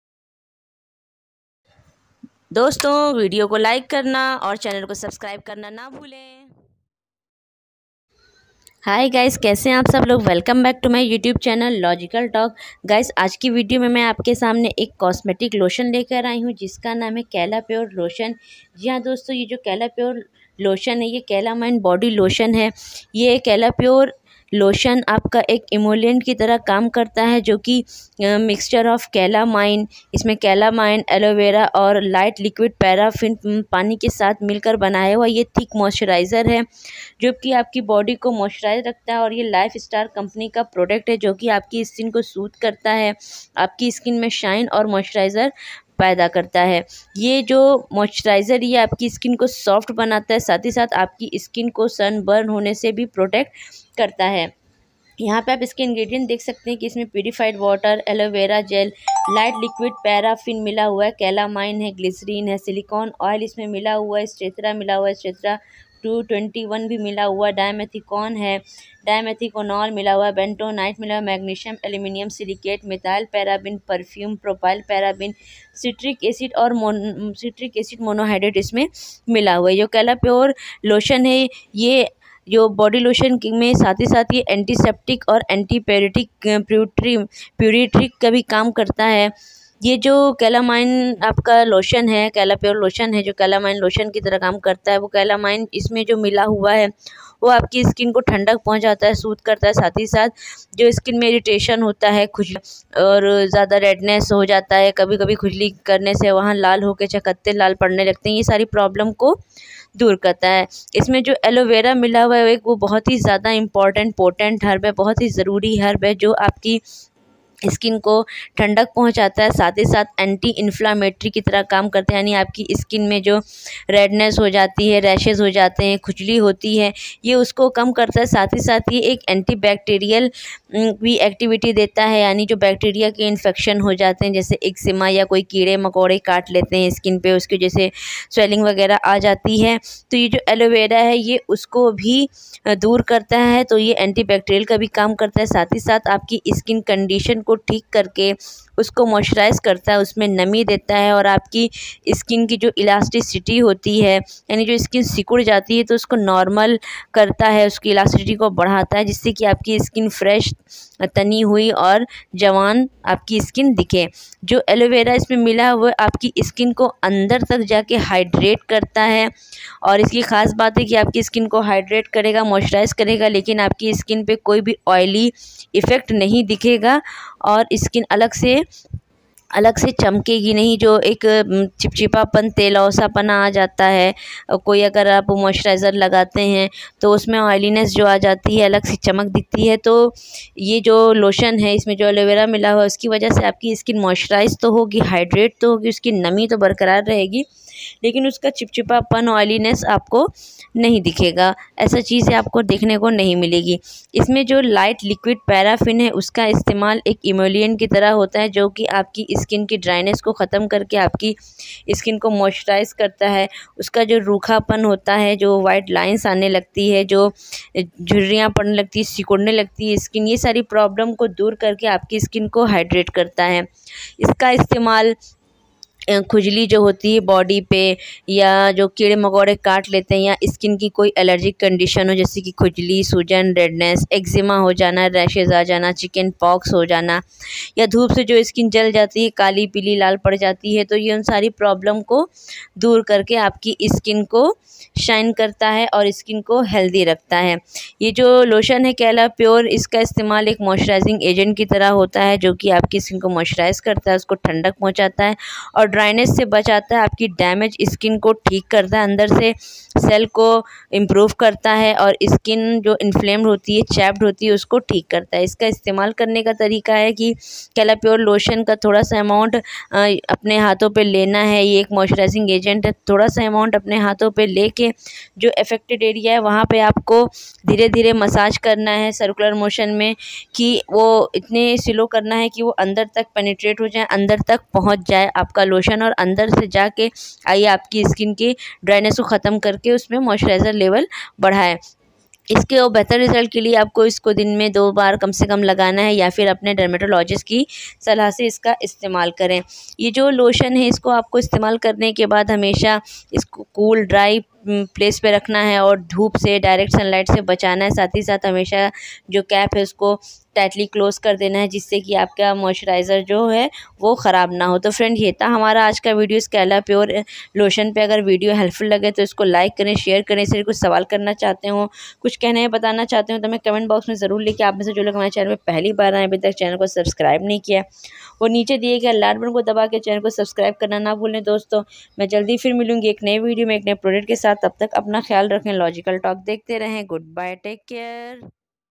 Calapure a lotion | कितने प्रकार के skin सम्सया को ठीक करता है
Wlcm 2 लॉजिकल/टॉक

Calapure A Lotion बिना डॉक्टर के पर्चे द्वारा मिलने वाली एलोपैथिक दवा है, Calapure A Lotion के मुख्य घटक हैं कैलामाइन, एलोवेरा, पैराफिन तरल Calapure A Lotion की उचित खुराक मरीज की उम्र, लिंग और उसके स्वास्थ्य संबंधी पिछली समस्याओं पर निर्भर करती है। यह जानकारी विस्तार से खुराक वाले भाग में दी गई है।

Calamine soothes and protects irritated skin.
Aloe vera is very potent herb known for its skin soothing, anti inflammatory as well as antibacterial activity. It heals skin condition, moisturizes and improves the elasticity of the skin. Aloe Vera hydrates the dry skin area without leaving it oily or shiny.
Light liquid paraffin is used as emollient to prevent dryness of the skin.

बहुत सी skin सम्सया का समाधान?
khujali se Rahat
skin problem se chhutkara
Dhup se kharab Hone Wali skin pe lagaen
bahut se skin problem ko theek karta hai

koi bhi Dava Lene se pahle Ek Bar Jarur Apne doctor se Rai Lena chahie

Useful in treatment of:
Itching and Pruritus
Insect bites
Skin allergy, Eczema and rashes
Chicken pox
Sunburn

Koi bhi Dava Lene se pahle Apne doctor se ek bar consult karna bahut jaruri hai